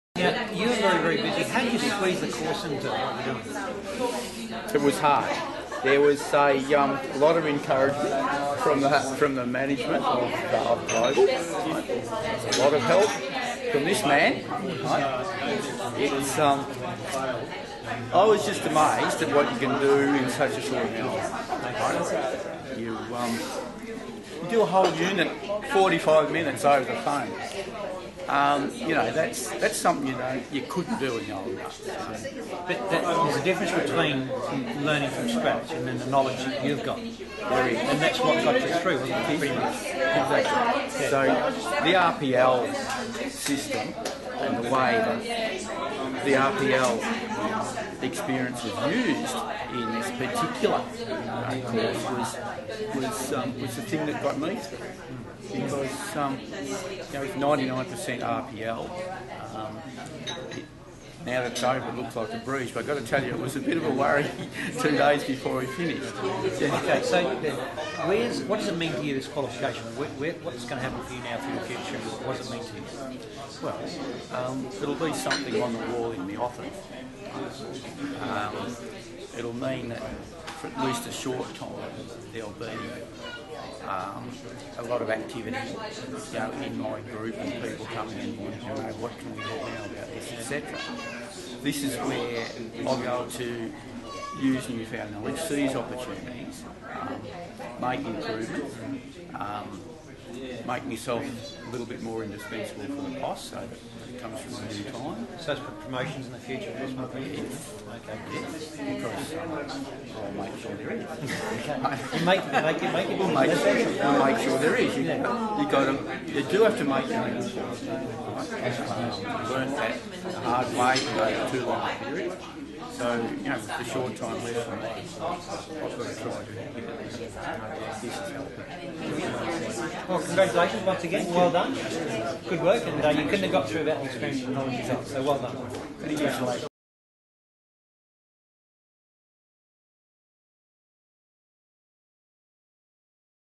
RII50420 | Diploma of Civil Construction Management | Global Training Institute Testimonial
Global Training Institute speaks with Henry Nightingale at his Graduation Ceremony at a top resort on the beautiful Sunshine Coast in Queensland. Henry completed his Diploma of Civil Construction Management. Discover how Henry and hundreds of others are able to successfully complete their higher level qualifications online and hear about the strong support offered by the team at Global Training Institute.